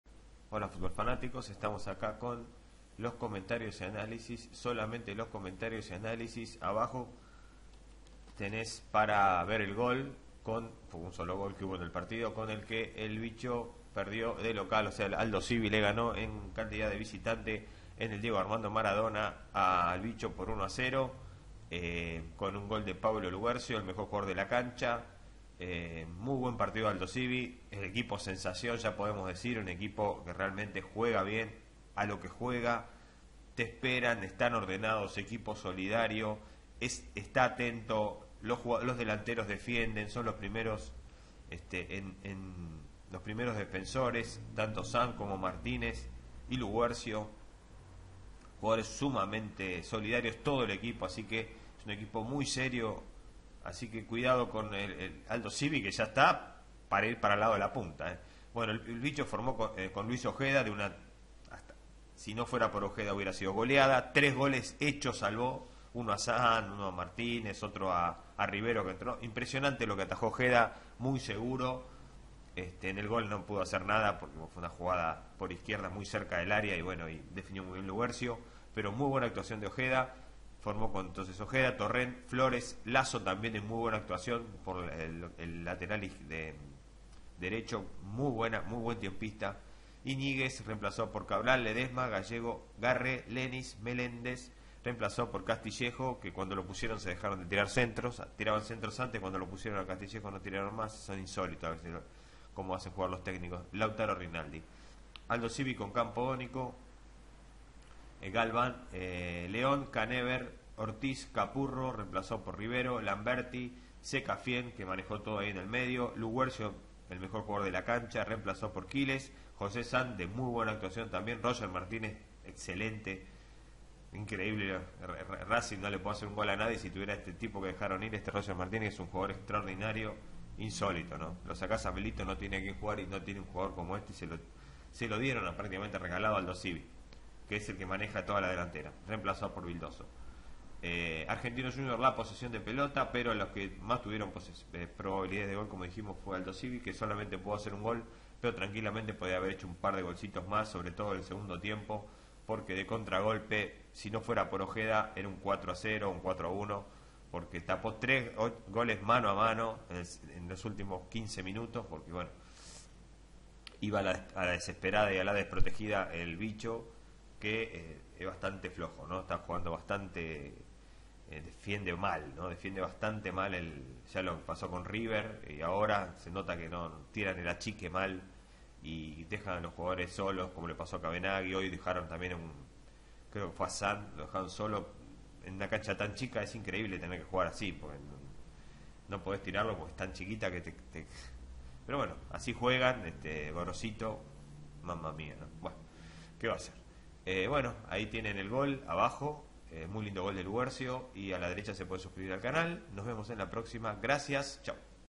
Argentinos Jrs 0 - 1 Aldosivi de Mar del Plata - Fecha 11 - Resumen del Match y goles
Estamos aqui